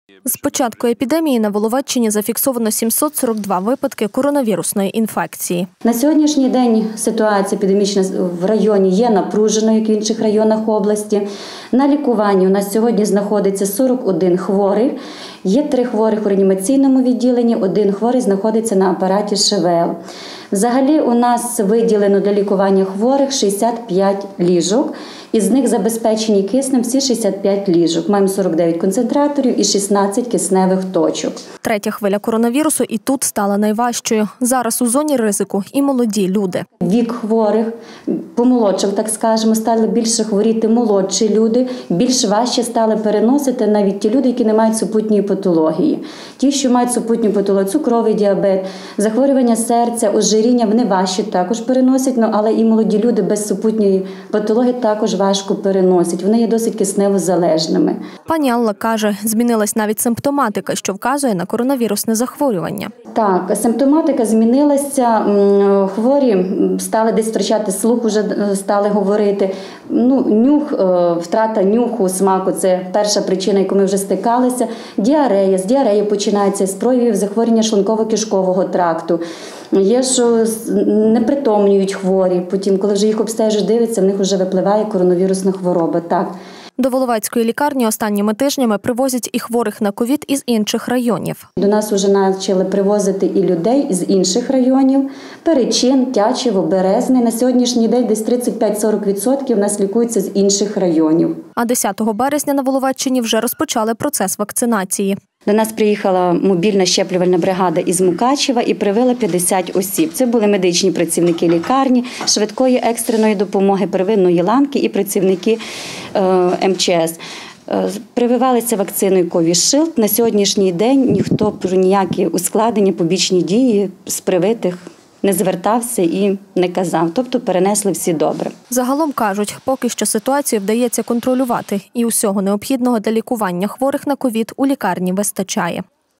Захворюваність на ковід та вакцинація
З початку епідемії на Воловеччині зафіксовано 742 випадки коронавірусної інфекції.Третя хвиля коронавірусу і тут стала найважчою. Зараз у зоні ризику і молоді люди.Пані Алла каже, змінилась навіть симптоматика, що вказує на корона вірусне захворювання.До Воловецької лікарні останніми тижнями привозять і хворих на ковід з інших районів.10 березня на Воловеччині вже розпочали процес вакцинації. Загалом кажуть, поки що ситуацію вдається контролювати, і усього необхідного для лікування хворих на ковід у лікарні вистачає.